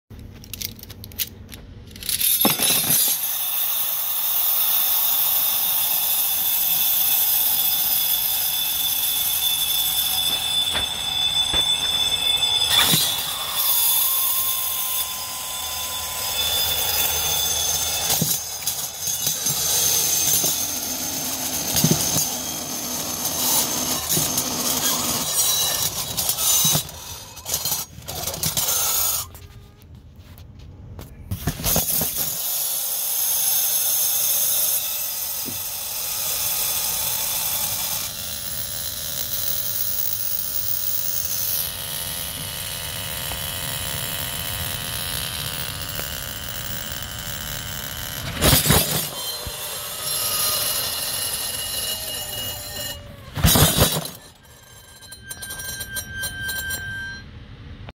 Throwing a bunch of spoons into mediocre dry ice
I throw spoons into a container of dry ice